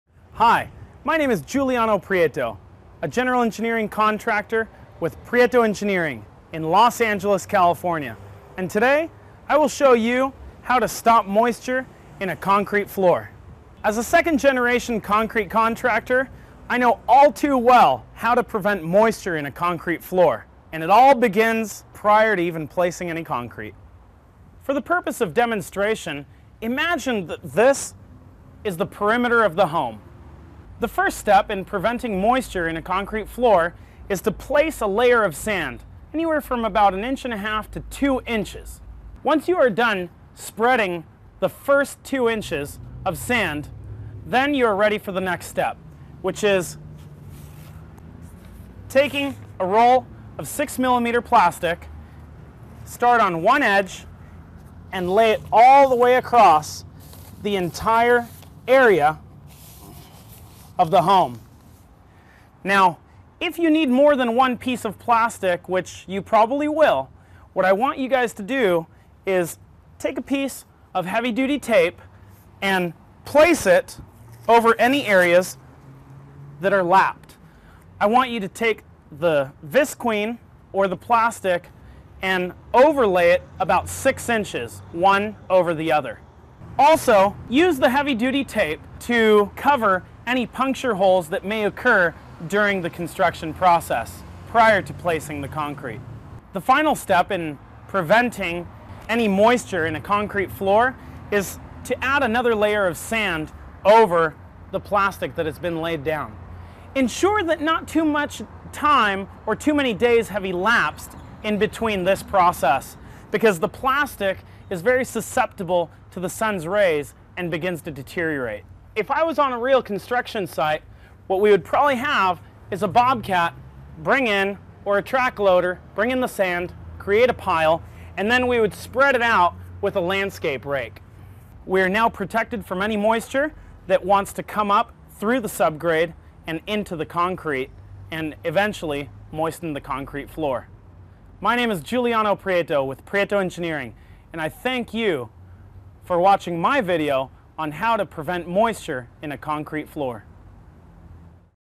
How to Stop Moisture in a Concrete Floor
How to Stop Moisture in a Concrete Floor. Part of the series: Concrete & Masonry. If moisture seeps into concrete, it can damage and weaken your floor. Stop moisture in its tracks with help from a licensed engineering contractor in this free video on concrete and masonry.